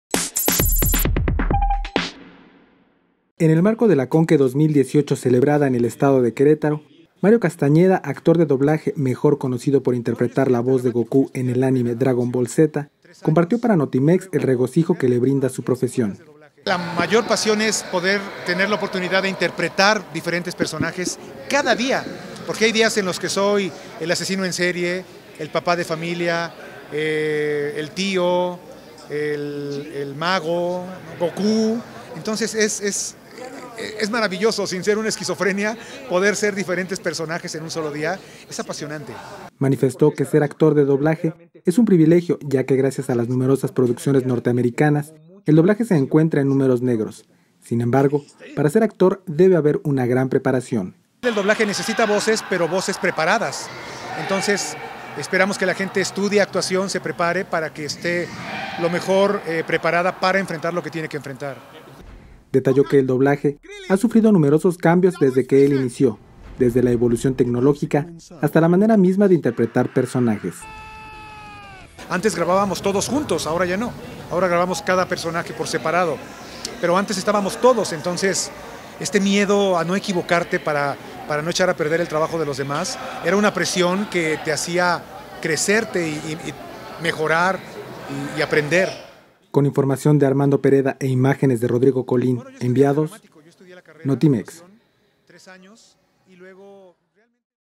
El doblaje es una profesión apasionante: Mario Castañeda
En el marco de la CONQUE 2018 celebrada en el estado de Querétaro, Mario Castañeda, actor de doblaje, mejor conocido por interpretar la voz de Goku en el anime Dragon Ball Z, compartió para NOTIMEX el regocijo que le brinda su profesión.